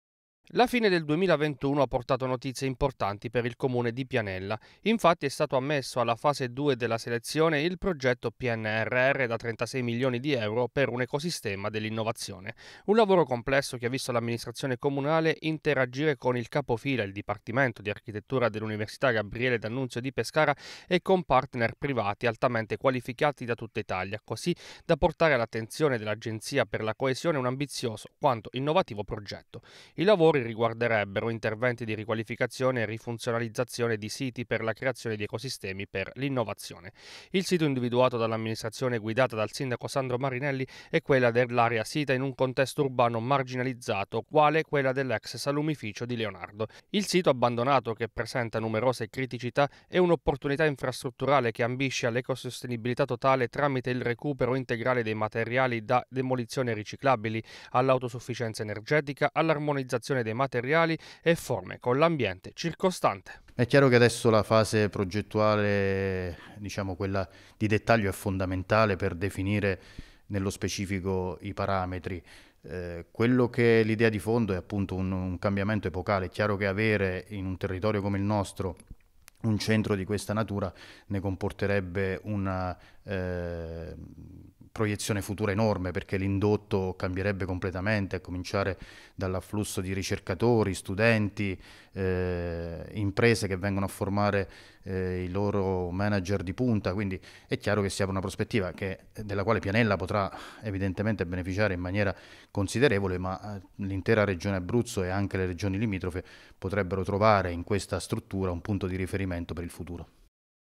Pianella: progetto Pnrr da 36 milioni per un ecosistema dell’innovazione
Trasformare un area in abbandono per farne un centro di ricerca e formazione altamente qualificato con un budget stimato in 36 milioni di euro. Il progetto, che vede tra gli altri il Comune di Pianella, si basa su tre asset principali: la ricerca e la formazione di alto livello in ambito di mobilità sostenibile, di digitalizzazione della P.A. e di processi innovativi per l’industria 4.0